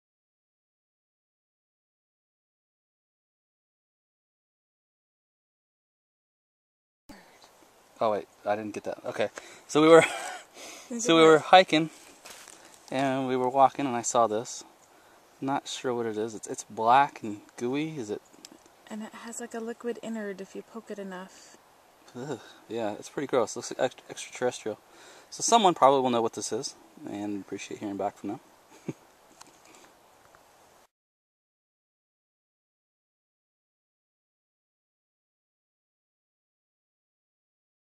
Space Slime
Strange substance found while hiking Calaveras South Grove trail on September 16, 2012. It was on a fallen tree right near the trail, approximately 3 feet up. It seemed to liquify upon touch.
I have found photos of similar substance by typing "black slime mold" into Google image search.